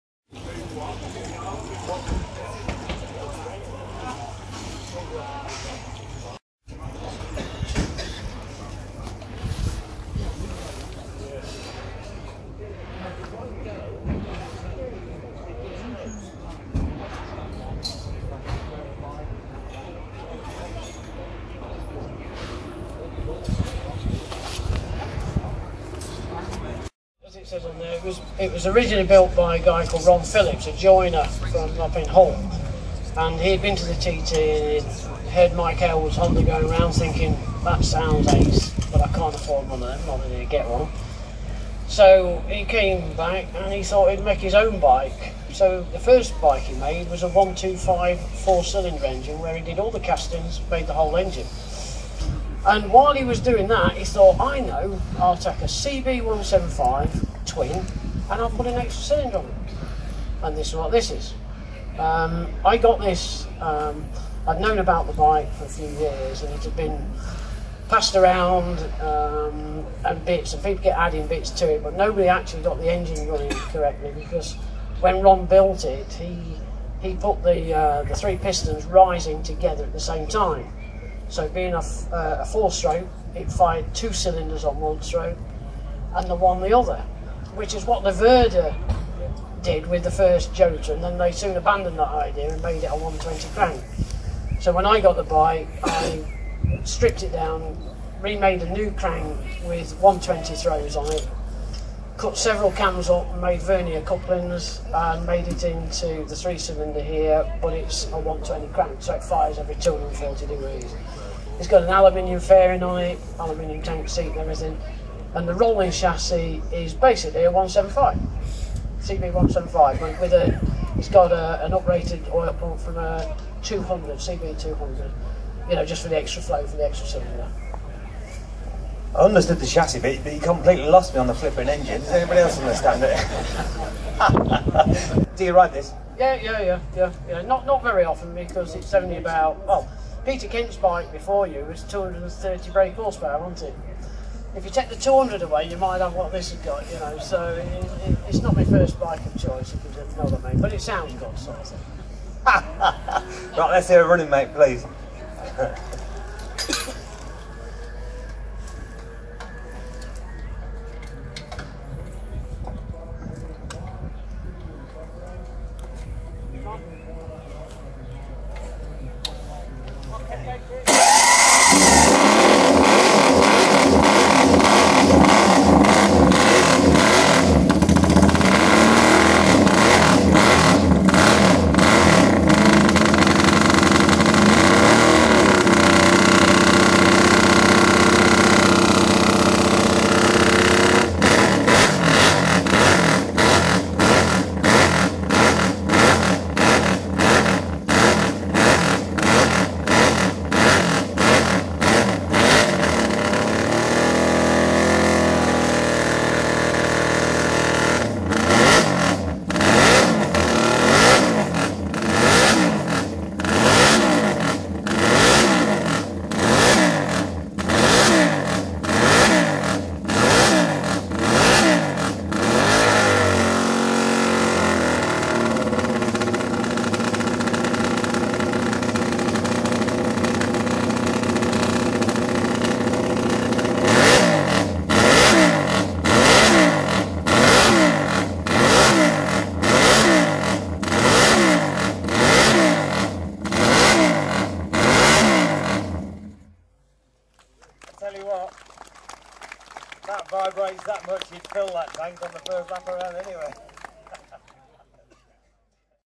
Honda 260 triple... Shed built engineering
This Honda 260cc three cylinder engine was created from spliting two CB175 engines to make this fantastic piece of engineering